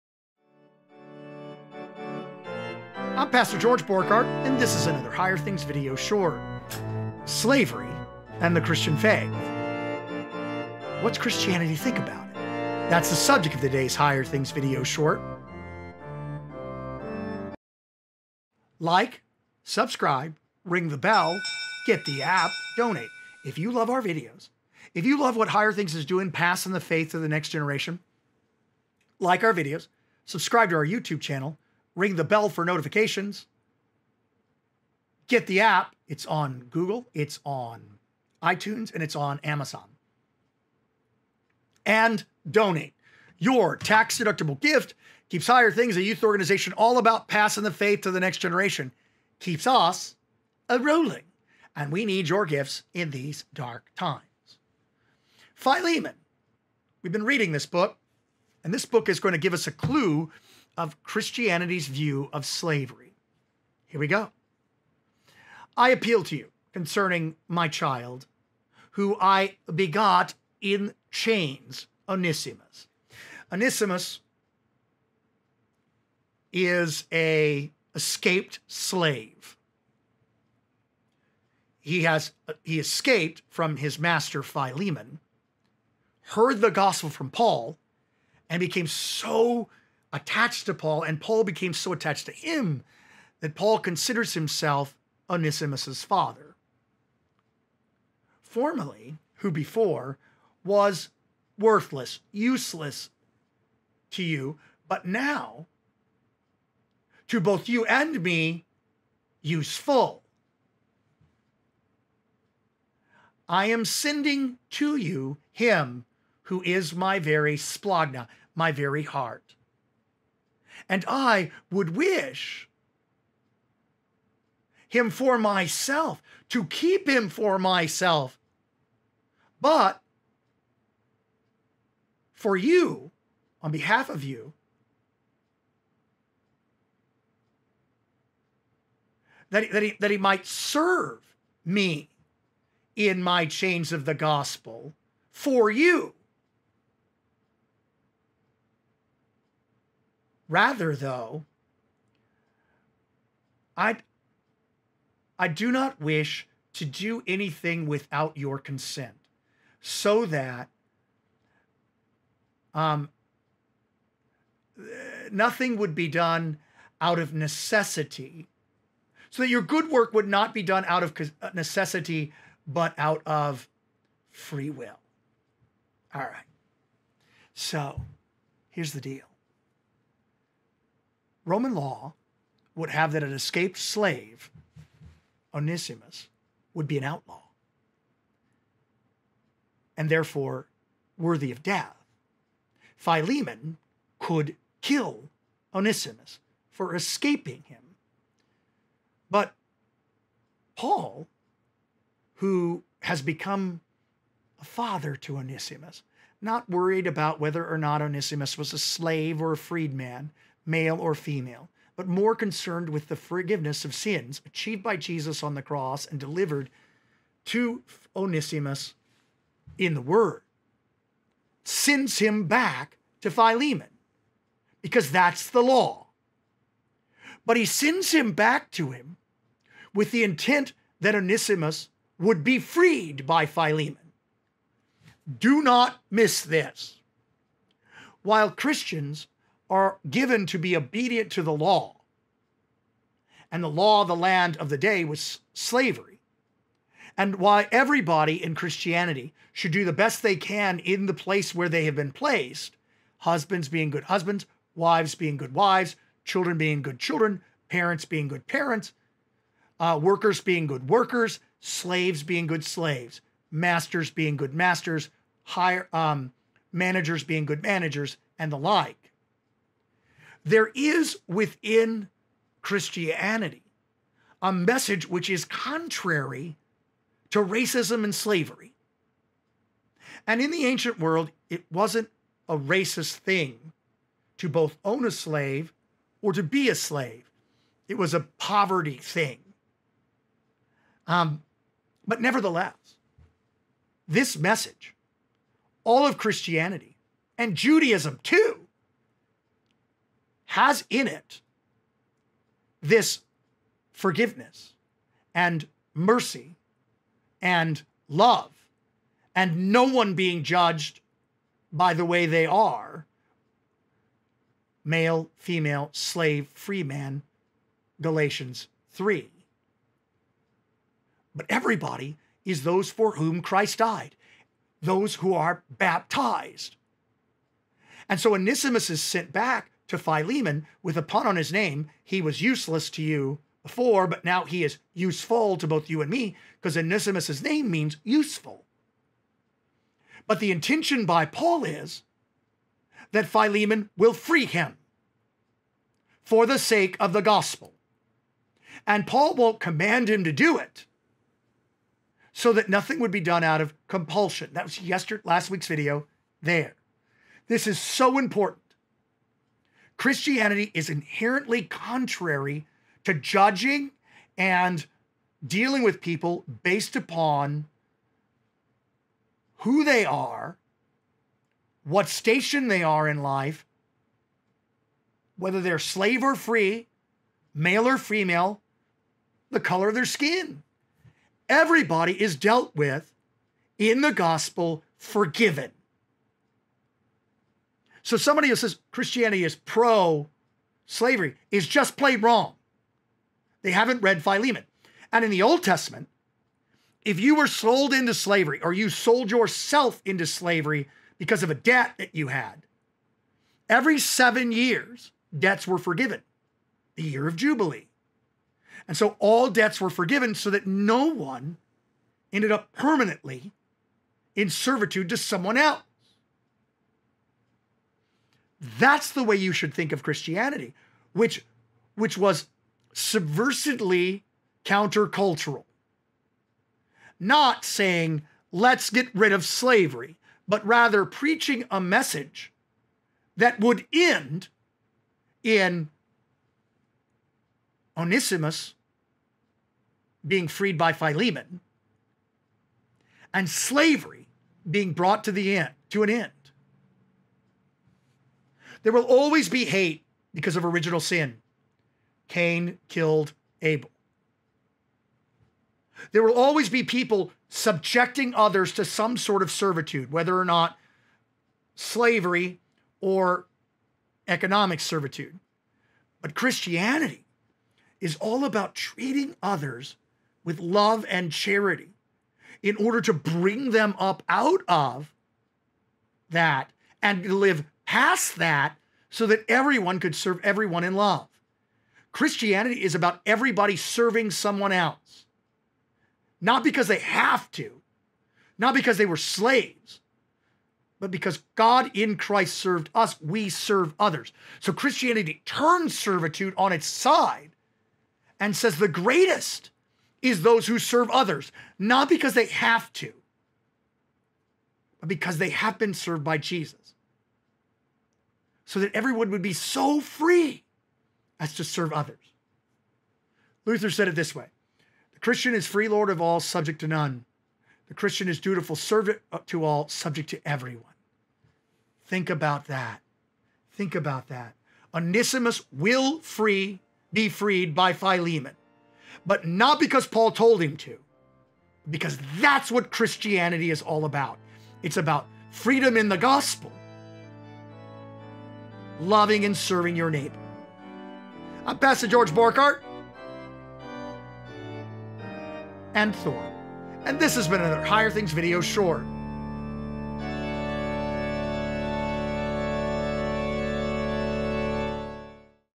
0618 Philemon and Christianity's Answer To Slavery - A Higher Things® Video Short
Even in ancient world where slavery abounded Christianity positioned itself and its message as antithetical to the practice. The Gospel teaches love and charity towards people of all backgrounds, races, and economical class. This message is antithetical to slavery today, and it was antithetical to slavery back in the days of Paul.